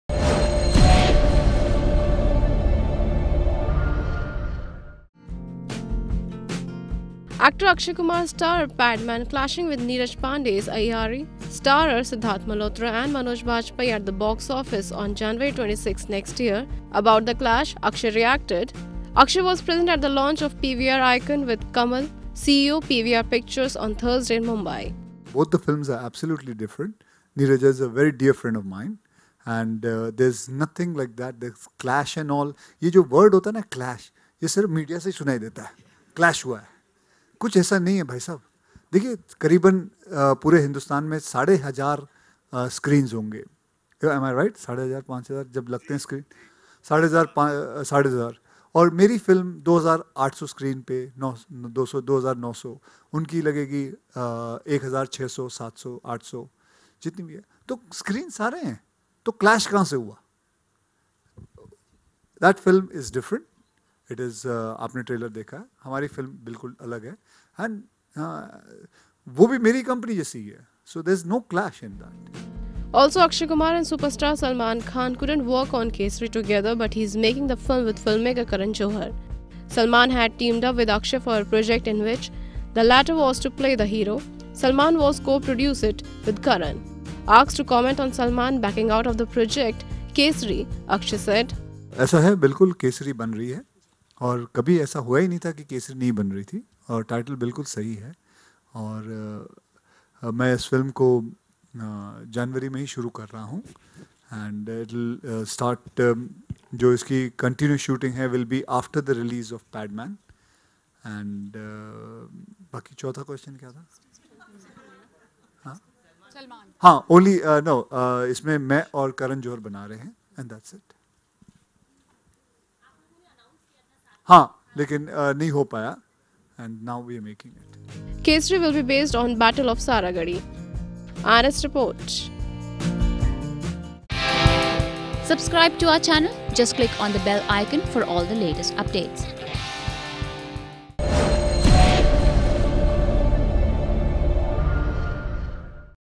Akshay REACTS on ‘Padman’ CLASH with ‘Aiyaary’
Actor Akshay Kumar starrer "Padman" clashing with Neeraj Pandey's "Aiyaary" starrer Sidharth Malhotra and Manoj Bajpayi at the box office on January 26 next year about the clash Akshay reacted.  Also, Akshay Kumar and Superstar Salman Khan couldnt work on "Kesari" together but he is making the film with filmmaker Karan Johar. Salman had teamed up with Akshay for a project in which the latter was to play the hero. Salman was to co-produce it with Karan. "Kesari" will be based on Battle of Saragarhi